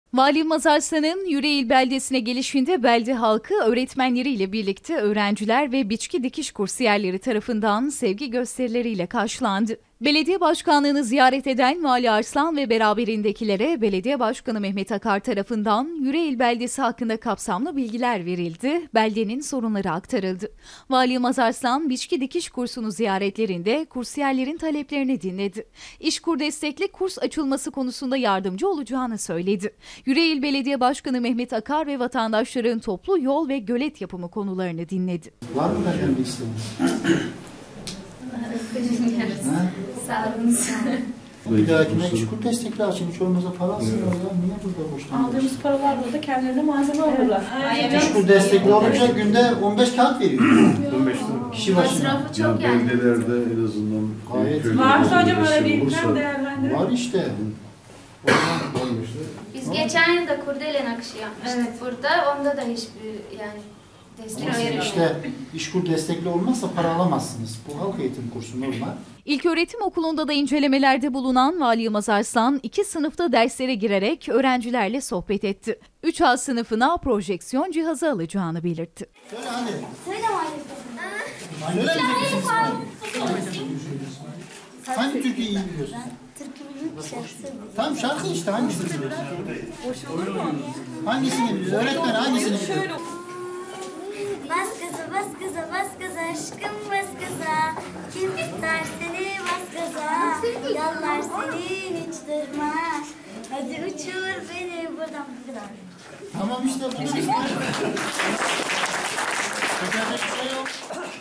Vali Yılmaz Arslan, Yüreğil Beldesi'nde Büyük Bir Sevgi Gösterisi ile Karşılandı...
Sındırgı gezisi esnasında Yüreğil Beldesi'ne de uğrayan Vali Yılmaz Arslan, belediye başkanı ve belde halkı tarafından büyük bir sevgi gösterisi ile karşılandı. Vali Yılmaz Arslan, Yüreğilliler'in sorunları ile yakından ilgilendi ve not tutarak bilgi aldı.